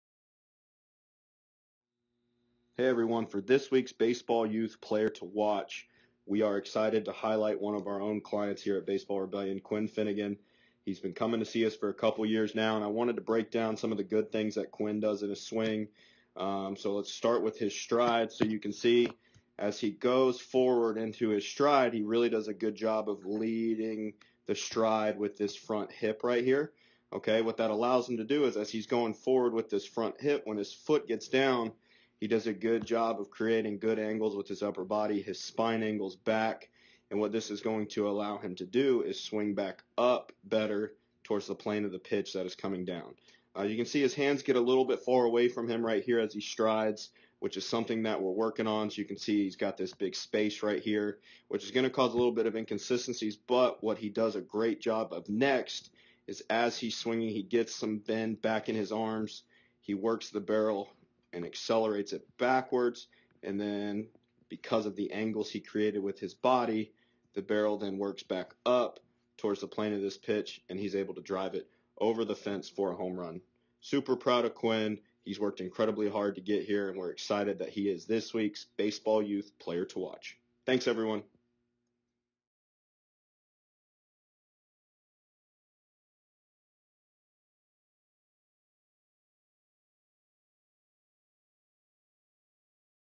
Quinn Finnegan | Baseball Youth | Swing Breakdown
Baseball Rebellion's Tyler Zupcic breaks down the swing of this week's Baseball Youth Player to Watch (& Baseball Rebellion client) Quinn Finnegan. Tyler points out how Quinn does a great job creating the proper angles with his body so he is able to use his large frame to drive the ball over the fence!